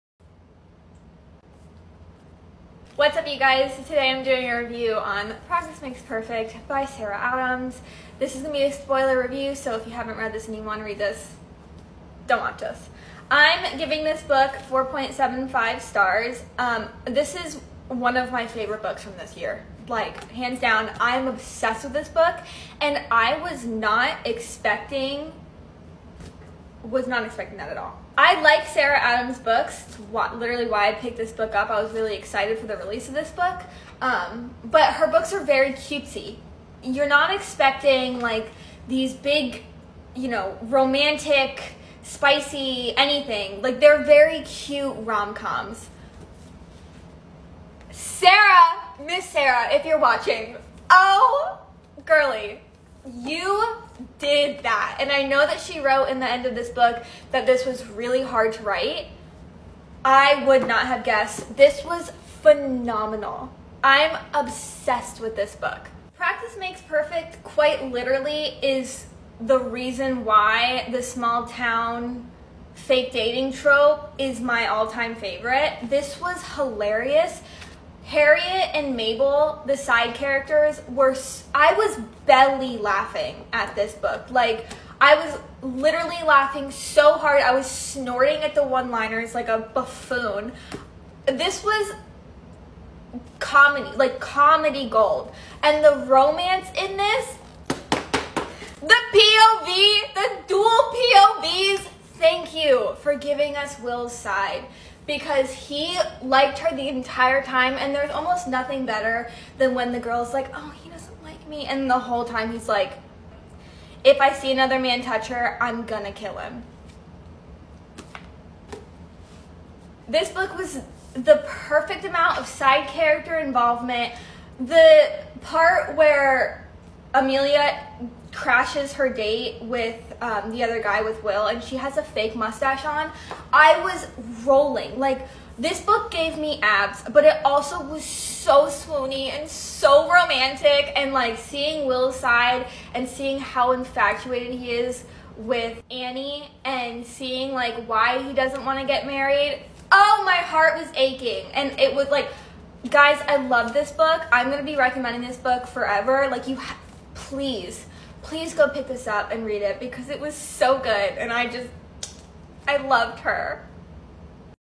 Practice Makes Perfect by Sarah Adams Book Review
Practice Makes Perfect Audiobook 🎧 by Sarah Adams (Preview)

Audiobook Summary
Practice Makes Perfect is a romantic comedy novel by Sarah Adams, set in Rome, Kentucky. The story follows Annie Walker, a flower shop owner, as she searches for her perfect match amid strict tutoring and practice dates. The novel features well-developed chemistry between Annie and her love interest, Will, and dual POVs. Set in the same town as Adams' previous work, When in Rome, it leaves readers feeling excited and excited. - By The Book Guide® Editor

Practice Makes Perfect Audiobook 🎧 by Sarah Adams